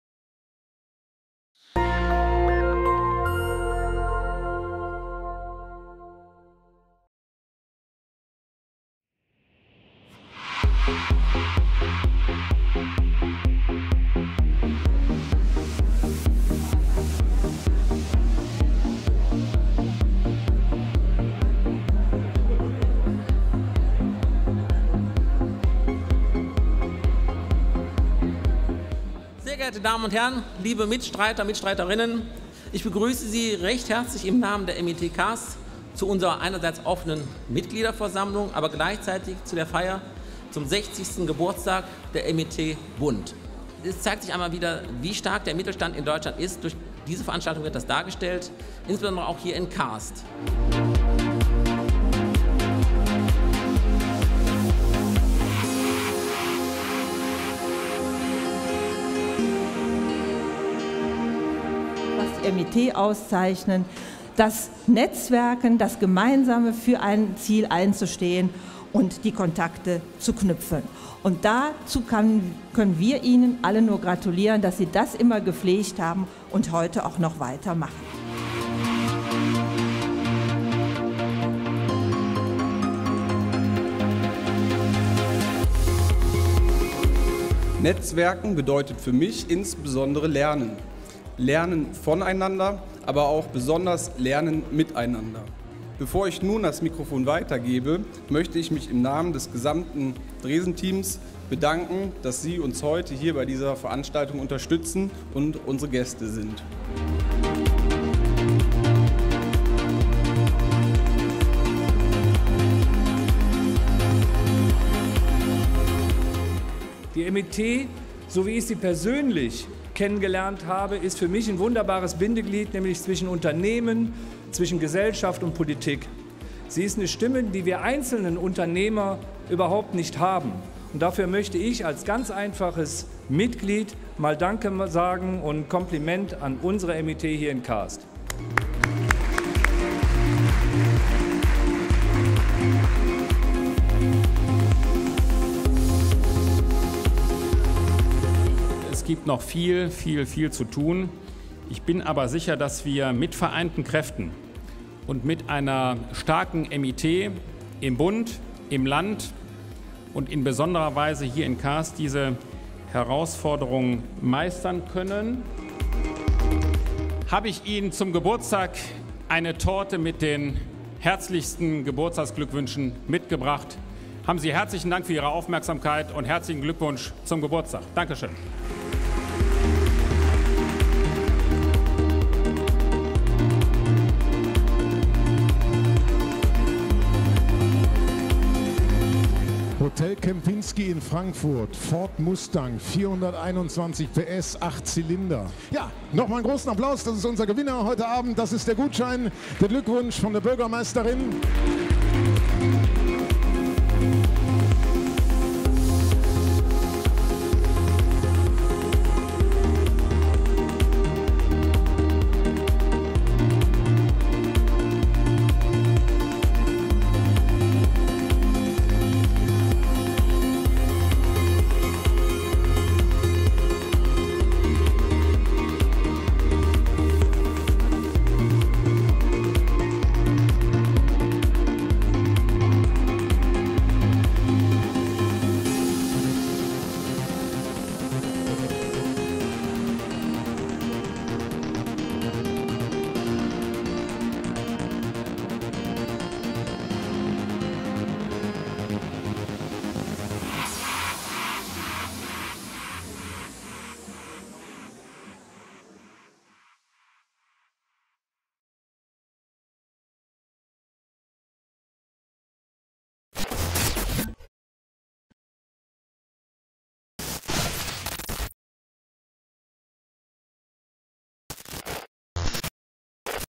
60 Jahre MIT Deutschland Feier im Stadtverband Kaarst im Autohaus Dresen in Kaarst
60Jahre MIT Deutschland
Feier im Stadtverband Kaarst im Autohaus Dresen in Kaarst